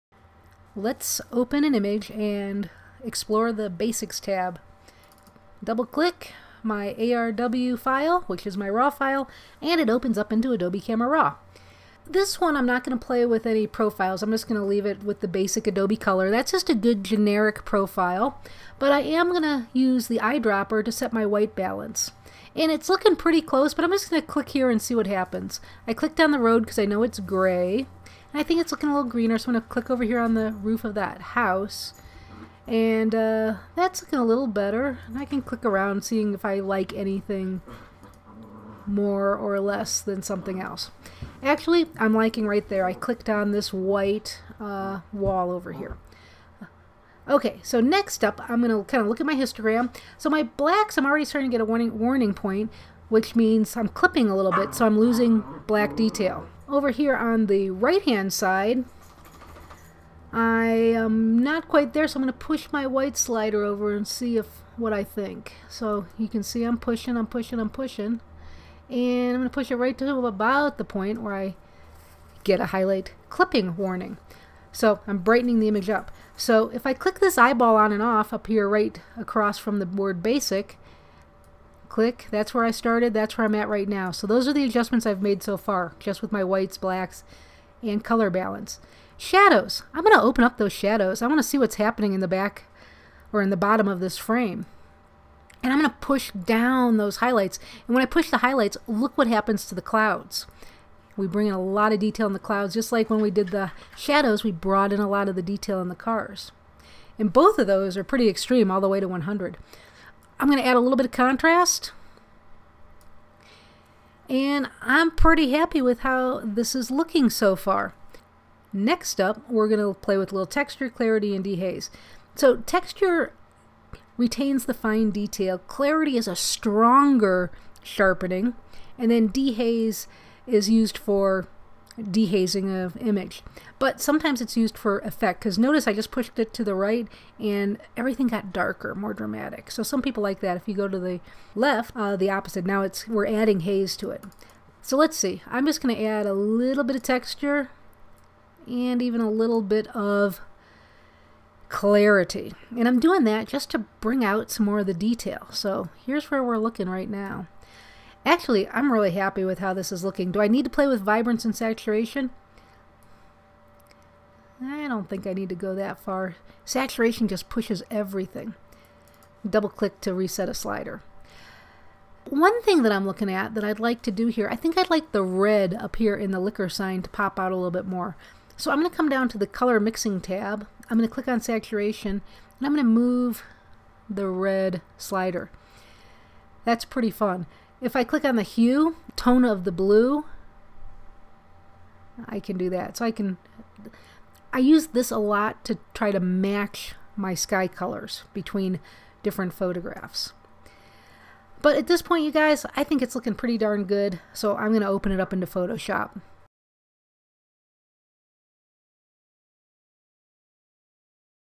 00007 Basics Panel 1
We process an image in ACR using the Basics Panel.
We show the Shadow slider in action with this image making a big recovery of the shadow detail. We also pop some red using the Saturation in the Color Mixer Panel.